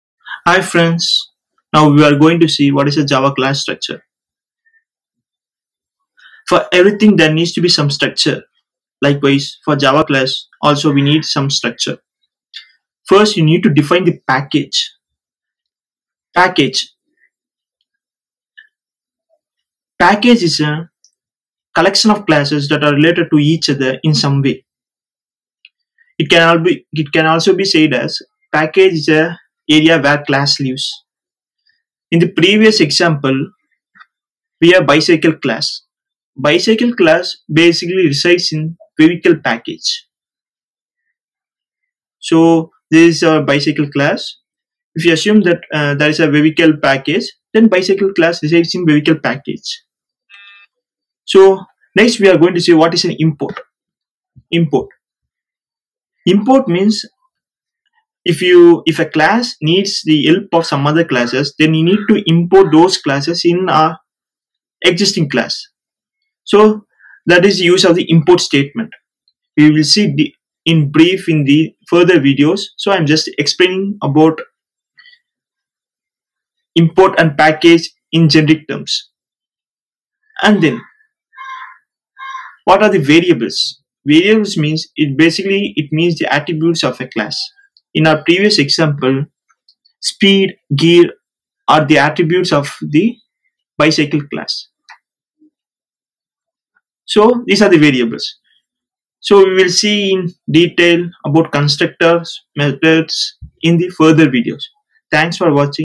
Java class structure (Java Tutorial)
Java Class declaration:
A class is a blue print for creating objects. You declare a class by specifying the class keyword followed by a non-reserved identifier like "Student", "Parent", and etc.

// class body